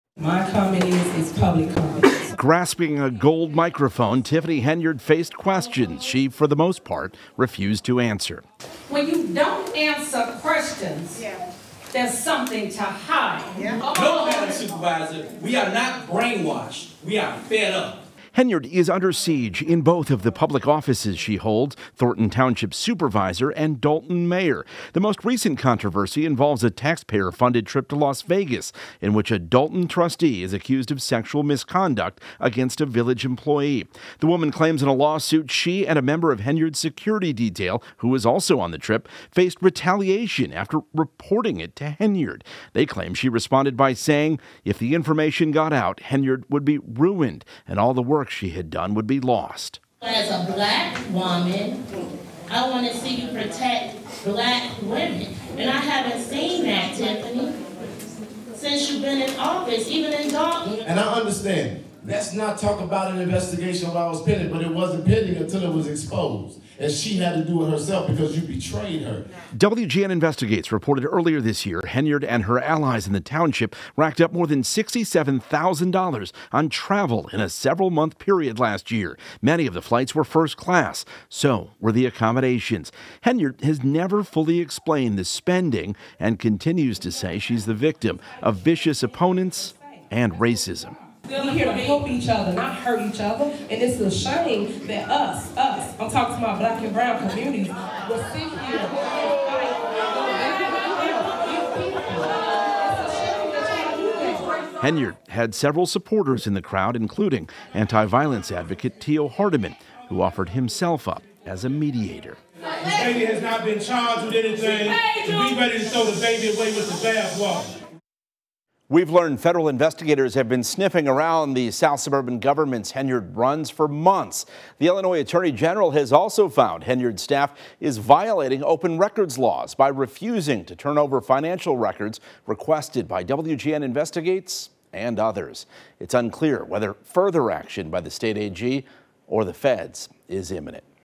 'We are fed up:' Tiffany Henyard faces angry residents demanding answers at community meeting
Frustrated residents showed up in force the Thorton Township meeting Tuesday demanding answers from Tiffany Henyard about everything from spending to a sexual misconduct claim against one of her allies. WGN Investigates' Ben Bradley reports on the story.